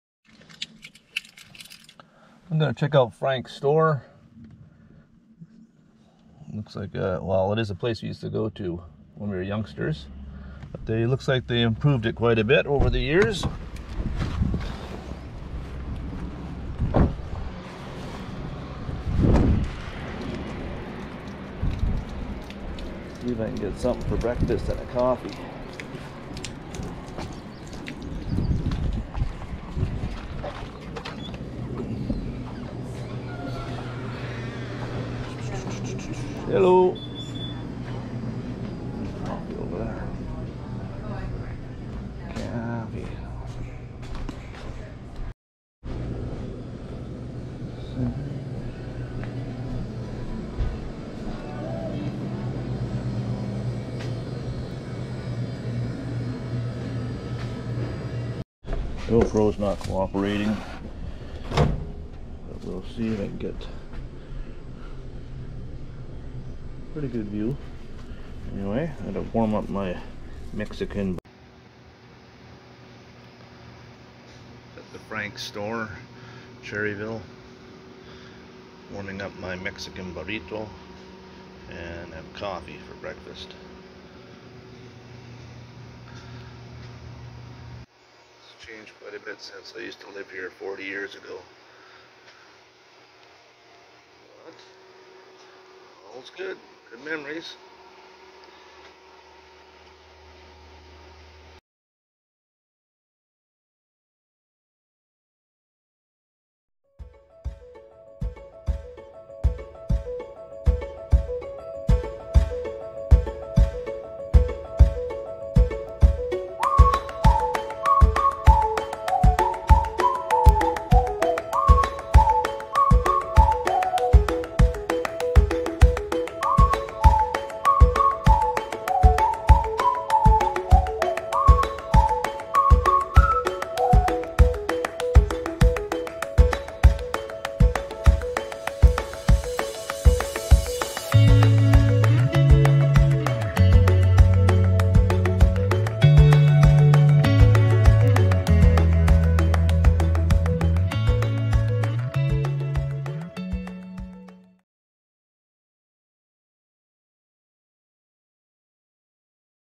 Frank's store, established in 1952.
An iconic landmark on Highway 6 in Cherryville, BC
Frank's General Store -

Locally owned and managed convenience store, established in 1952. An iconic landmark on Highway 6 in Cherryville, BC, offering fuel, groceries, deli items, ...

The Great Western Canada Bucket List

Amazon USA
Amazon Canada